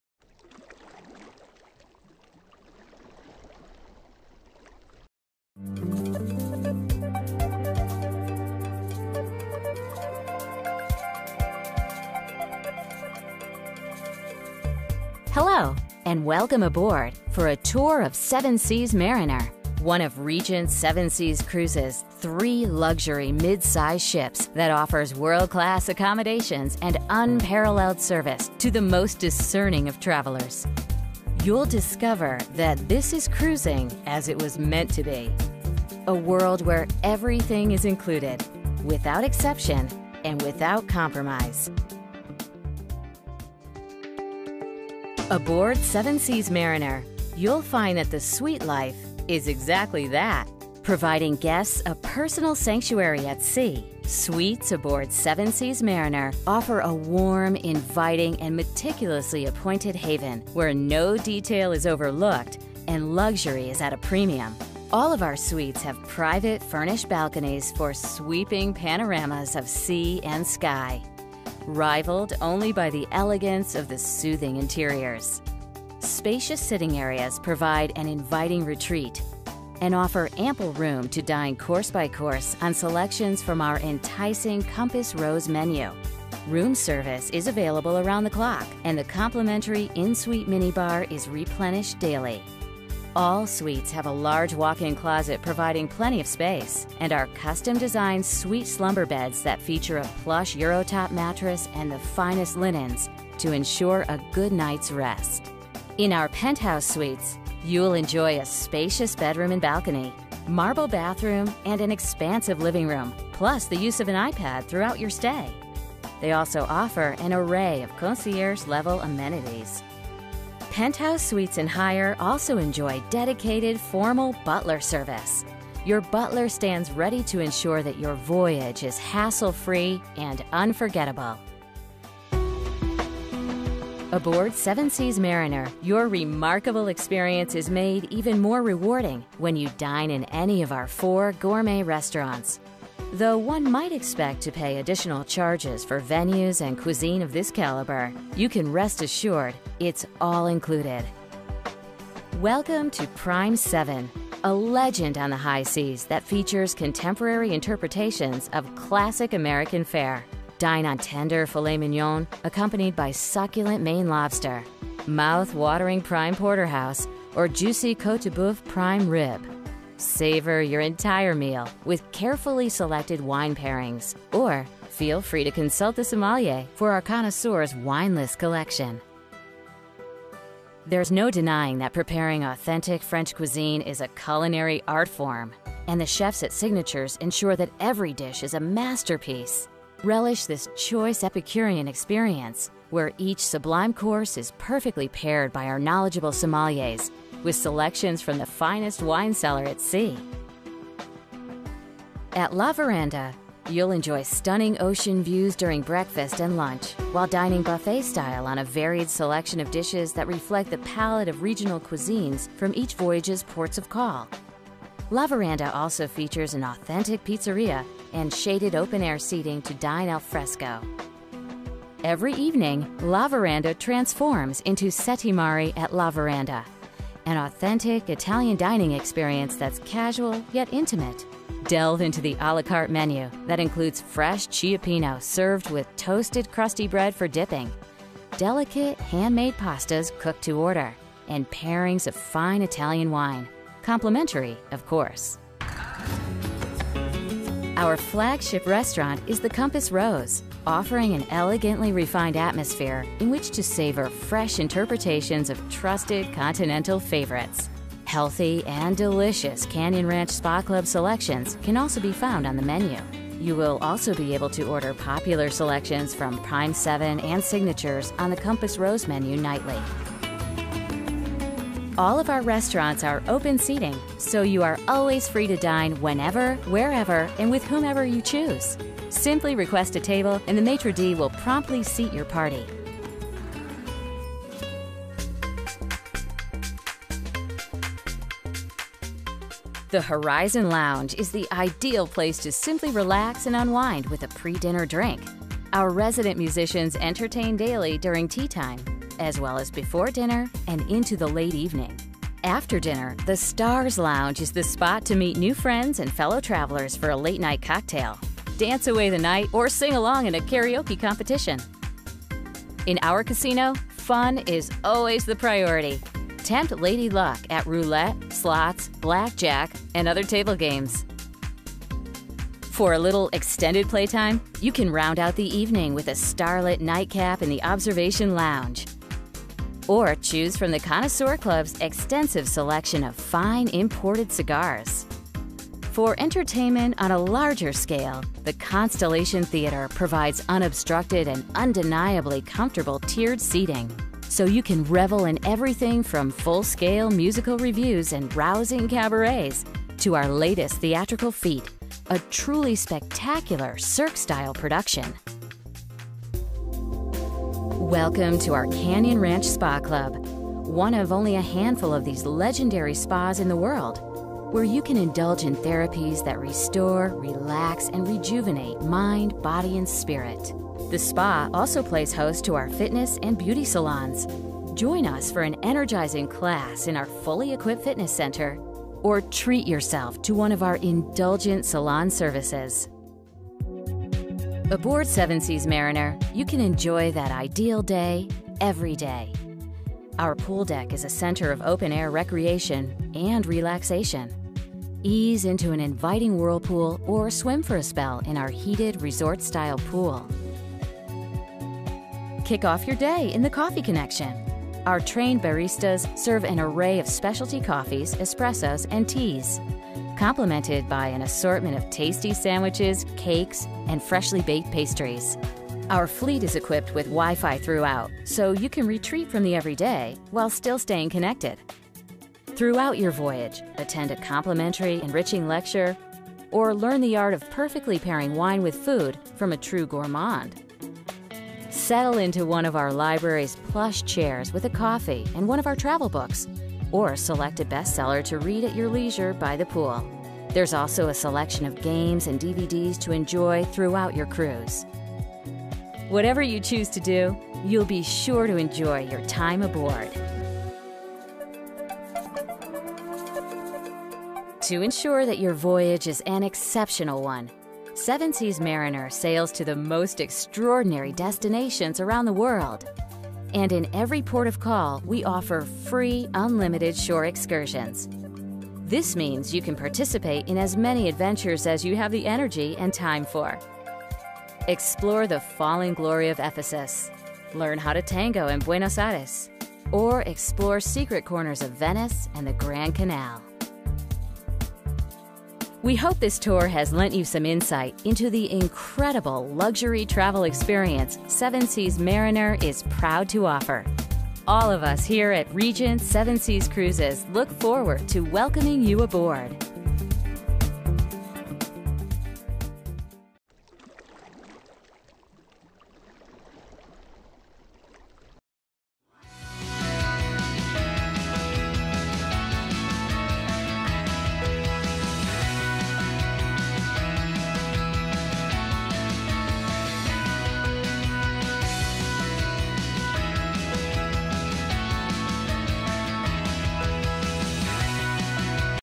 Regent Cruises - Welcome Aboard The Seven Seas Mariner | Planet Cruise
Regent Cruise's would like to Welcome you aboard The Seven Seas Mariner. Check out this video with a full tour and see what life on board is like.